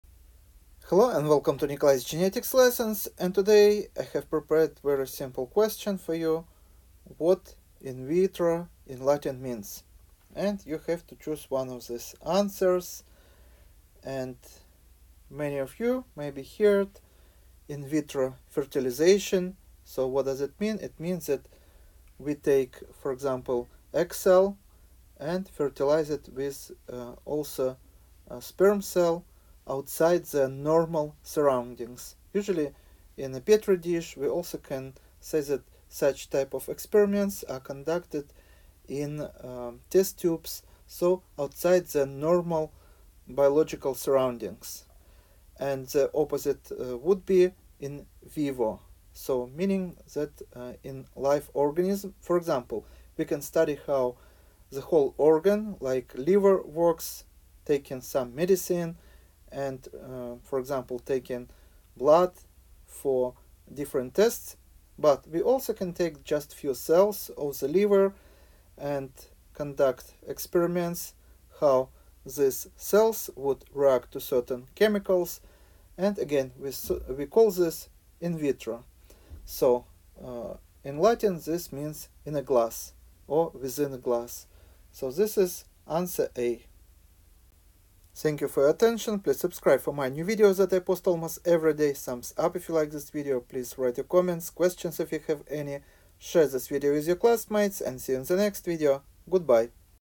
What does IN VITRO means?
In vitro studies are performed with microorganisms, cells, or biological molecules outside their normal biological context. Colloquially called "test-tube experiments", these studies in biology and its subdisciplines are traditionally done in labware such as test tubes, flasks, Petri dishes, and microtiter plates.